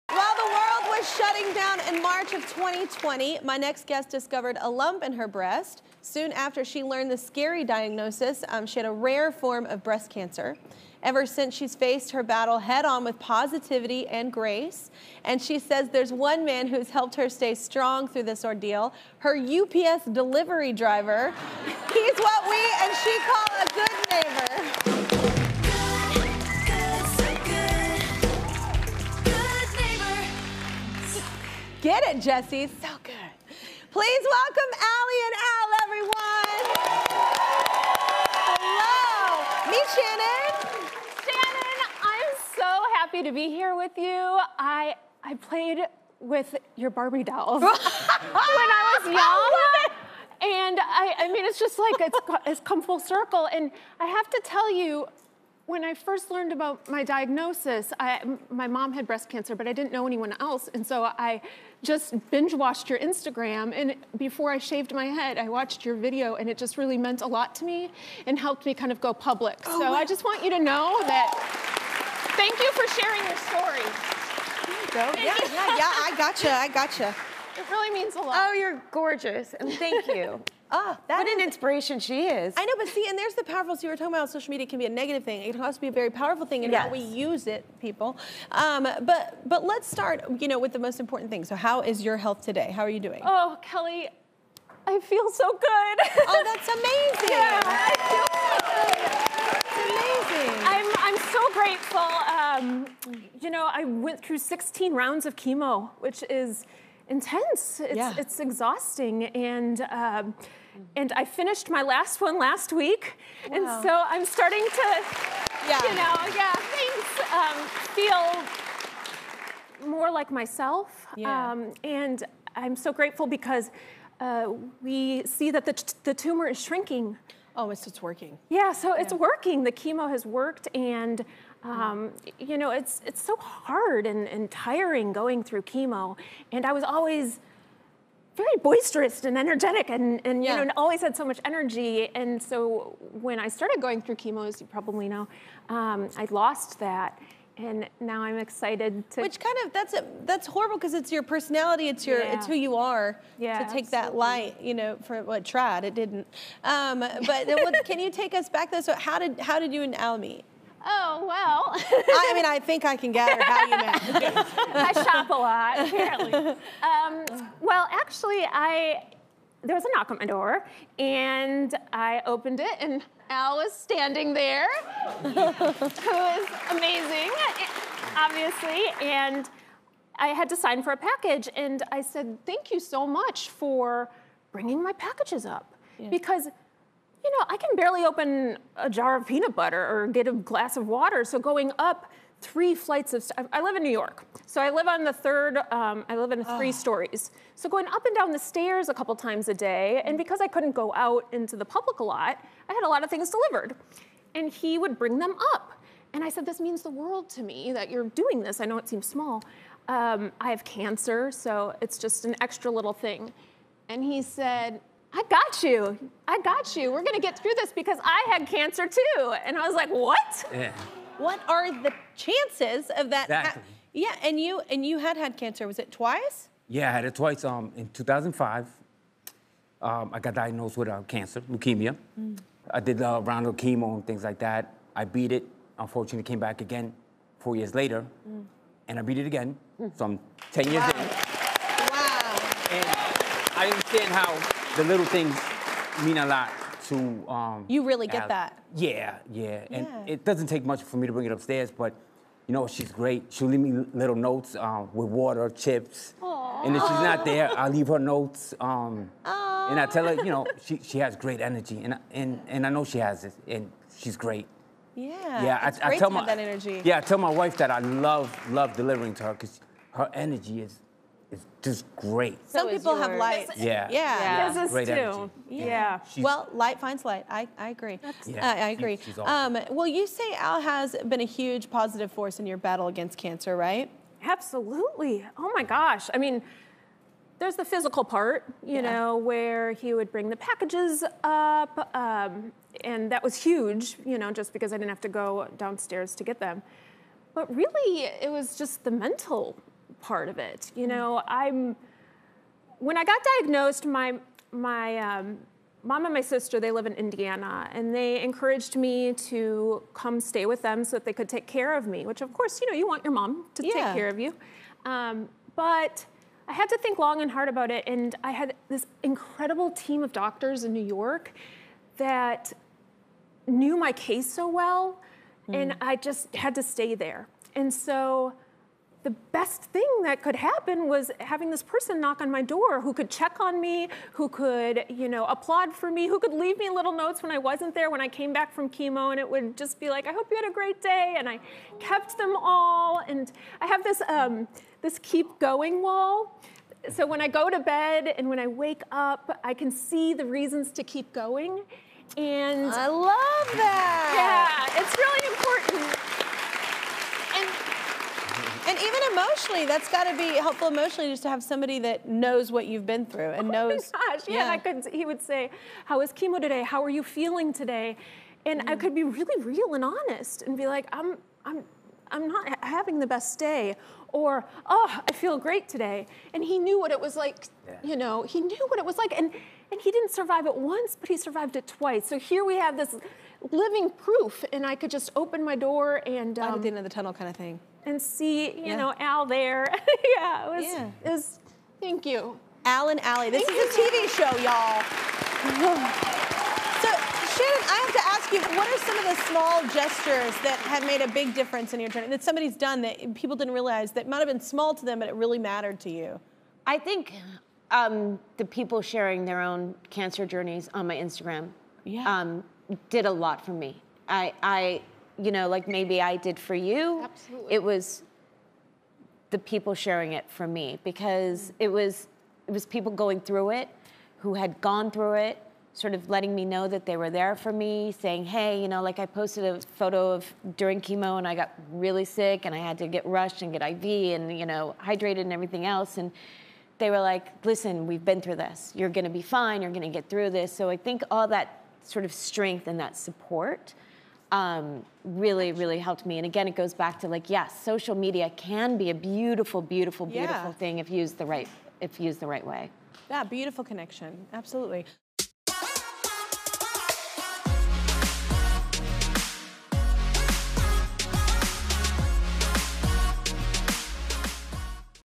Cancer Survivors' Unlikely Friendship Brings Shannen Doherty To Tears
Life has a weird way of giving us exactly what we need when we least expect it. For breast cancer survivor Allie, the one man who kept her strong throughout her battle just so happened to be her UPS delivery driver, and fellow cancer survivor, Al. Allie and Al reveal how they formed their unlikely friendship in a conversation that brings Shannen Doherty to tears.